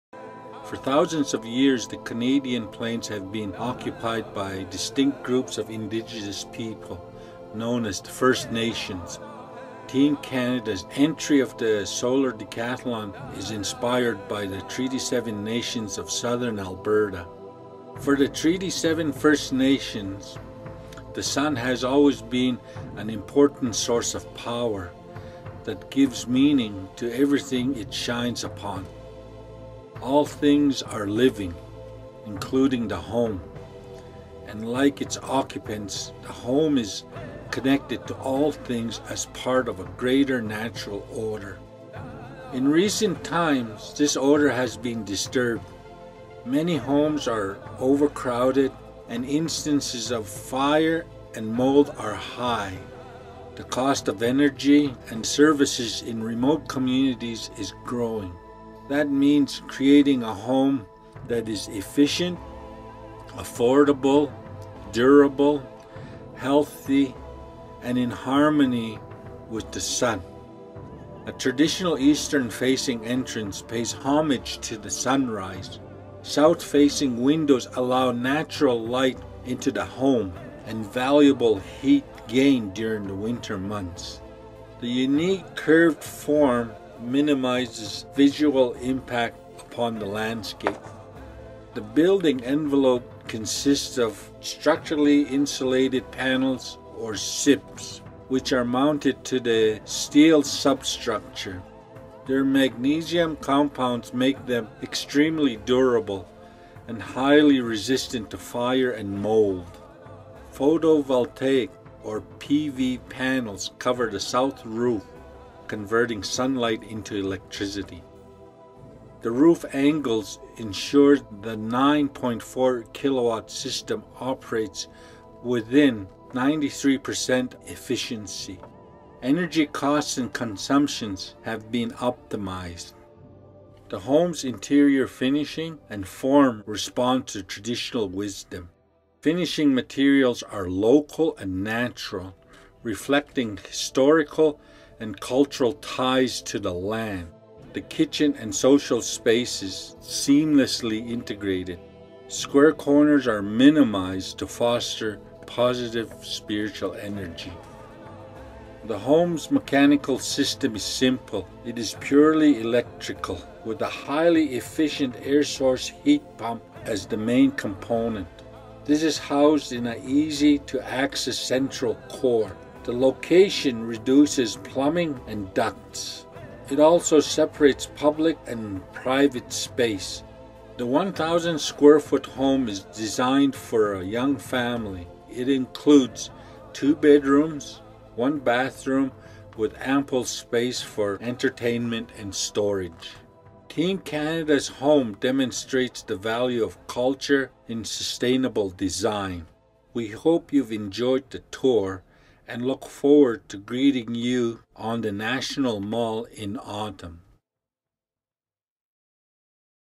Team Canada's Solar Decathlon 2011 Computer-Animated Walkthrough
All 20 Solar Decathlon 2011 teams produced a computer-animated walkthrough video to highlight their house design for the fall 2011 competition.